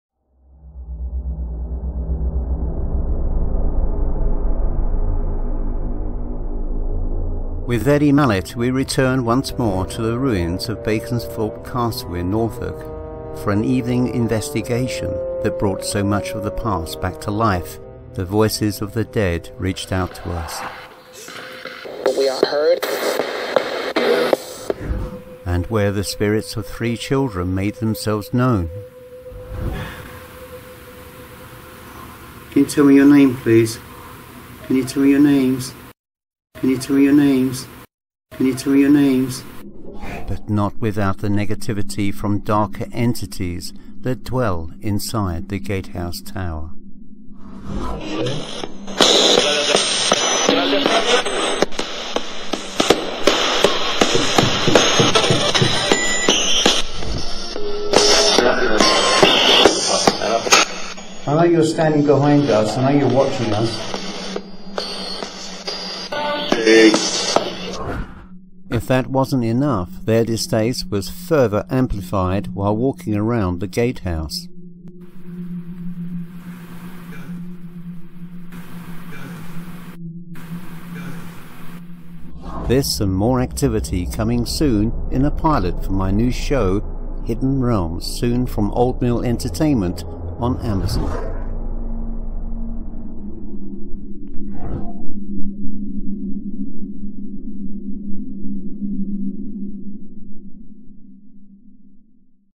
TRAILER FOR NEW PILOT SHOW - HIDDEN REALMS
Here is a trailer of a pilot for a new show, `Hidden Realms`.
The series explores rarely known strange stories about events or people,  or through investigations into the paranormal.
Not a conventional tv paranormal program, but neither a run of the mill history program either. More of a combination of both, but not always exclusively so as I seek to share the weird and the quirky through interesting dialog and unusual locations.
Join me as I visit a 15th century ruined castle in Norfolk, England where surprises await galore for both Chris Halton and fellow paranormal investigator, Eddie Mallett.